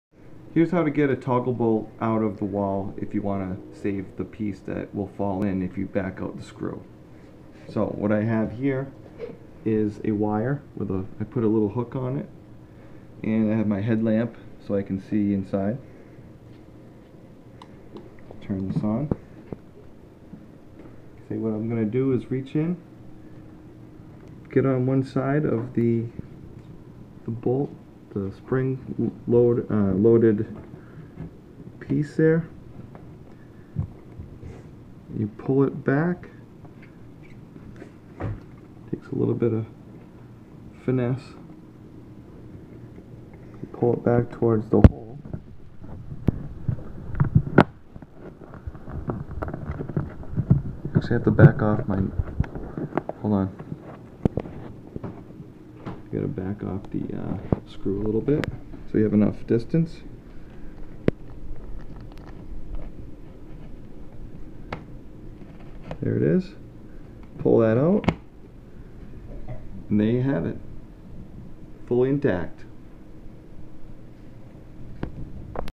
How to remove a toggle bolt without losing the anchor behind the wall
This is how to save your money and reuse your toggle bolts.  Be sure to know how long the screw is before you back off the anchor to the end. You will need to do that to make enough room for the anchor to fold in towards the hole. Good luck!